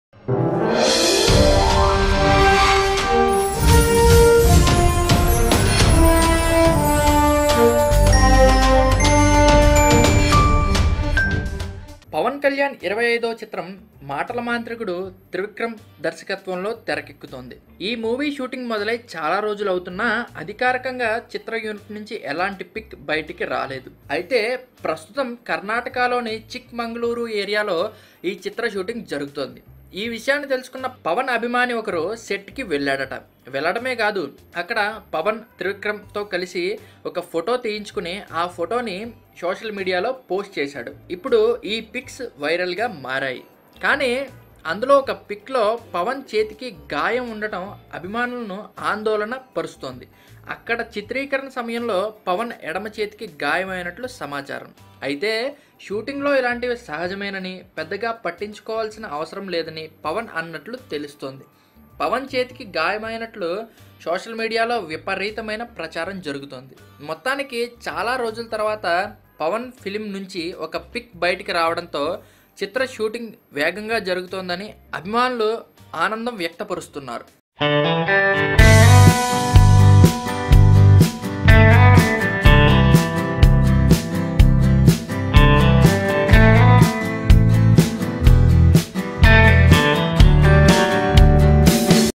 Pawan Kalyan Was Injured In Trivikram Movie Shooting || PSPK25 || Telugu Full Screen
Power Star Pawan Kalyan has been injured while shooting for the movie Agnathavasi. Watch latest videos for more updates.

MOVIES

NEWS

WEBSITES